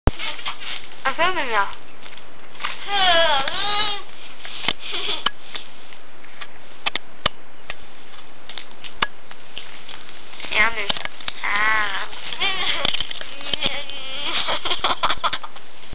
Ami Eating Marshmallow.
Ami eating a marshmallow at Brixham in Devon 2006.